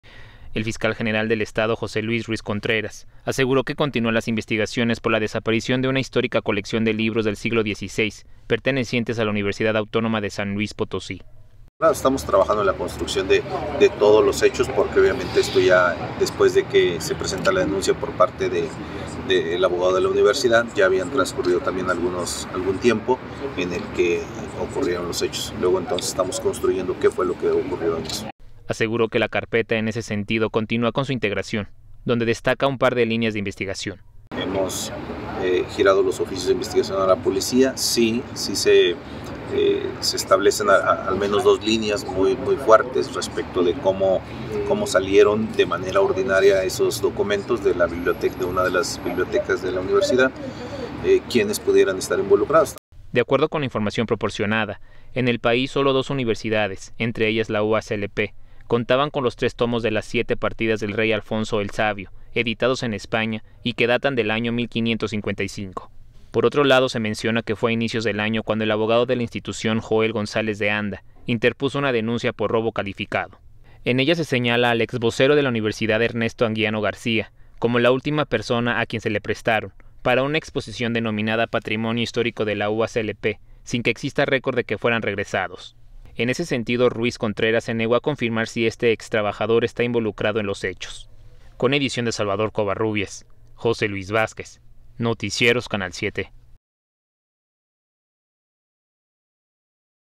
Mantiene Fiscalía dos líneas de investigación por robo de libros históricos de la UASLP
SLP.- El fiscal general del estado, José Luis Ruiz Contreras, aseguró que continúan las investigaciones por la desaparición de una histórica colección de libros del siglo XVI pertenecientes a la Universidad Autónoma de San Luis Potosí.

"Estamos trabajando en la construcción de todos los hechos porque obviamente después de que se presenta la denuncia por parte del abogado de la universidad, ya había transcurrido algún tiempo en el que ocurrieron los hechos, entonces estamos construyendo qué fue lo que ocurrió", dijo.

Aseguró que la carpeta en ese sentido continúa con su integración donde destaca un par de líneas de investigación.

"Hemos girado los oficios de investigación a la policía, sí se establecen al menos dos líneas muy fuertes respecto de cómo salieron de manera ordinaria esos documentos de una de las bibliotecas de la universidad y quienes pudieran estar involucrados", detalló.

De acuerdo con la información proporcionada, en el país solo dos universidades, entre ellas la UASLP, contaban con los tres tomos de Las Siete Partidas del Rey Alfonso el Sabio editados en España y que datan del año 1555.

Por otro lado, se menciona que fue a inicios de año cuando el abogado de la institución, Joel González de Anda, interpuso una denuncia por robo calificado.

En ella se señala al ex vocero de la universidad, Ernesto Anguiano García, como la última persona a quien se le prestaron para una exposición denominada "Patrimonio Histórico de la UASLP" sin que exista récord de que fueran regresados.

Sin embargo, Ruiz Contreras se negó a confirmar si este extrabajador está involucrado en los hechos.